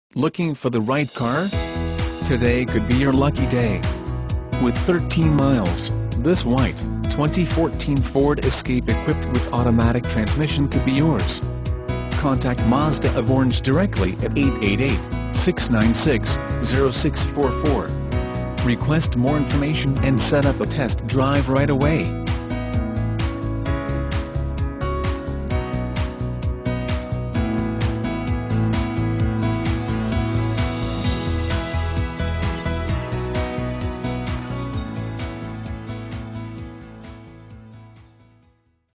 2014 Ford Escape Orange CA 00CD1771
2014 Ford Escape SE

Looking for the right car? Today could be your lucky day. With 13 miles, this WHITE, 2014 Ford Escape equipped with Automatic transmission could be yours. Request more information and set up a test drive right away.